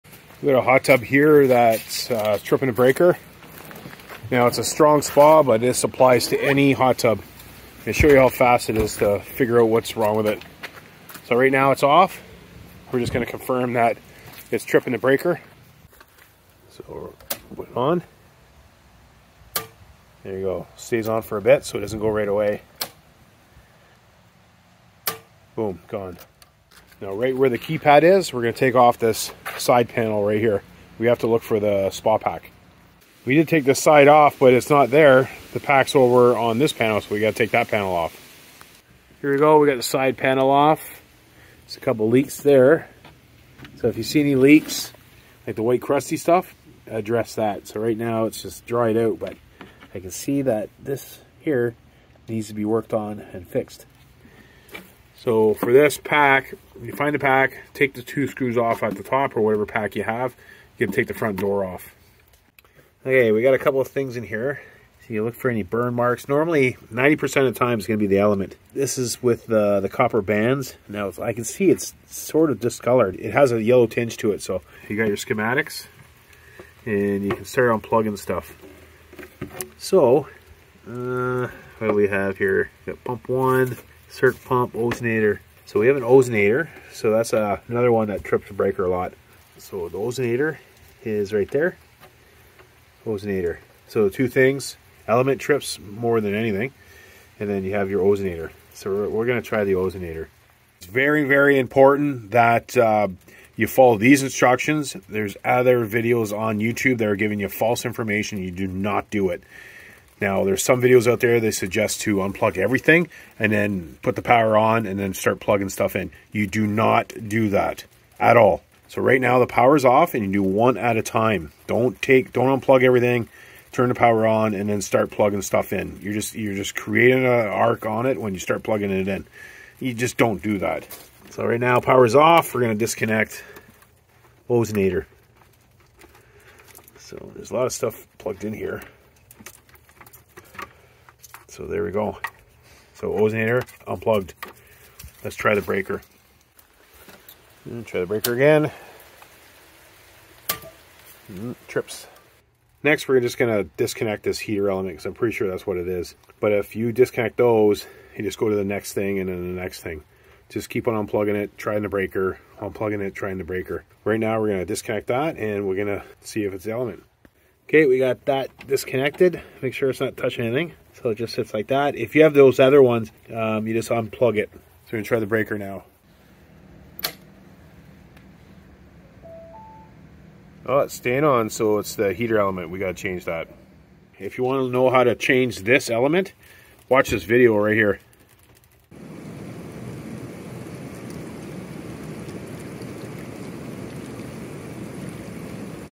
HOT TUB Trips the BREAKER / Hot Tub is Tripping the Breaker
This hot tub trips the breaker after about 3 seconds.  Follow this video to troubleshoot the reason your hot tub is tripping the gfci breaker.

5️⃣Pool and Hot Tub Wrench

7️⃣Pool and Hot Tub Silicone

8️⃣Hot Tub Heater Element 4KW

9️⃣Hot Tub Heater Element 5.5KW

👀 Legal Notice: Any electrical connected to your hot tub or swimming pool should be worked on by a licensed electrician or service technician.  Please call your local pool/hot tub Technician for a service call before doing any work yourself.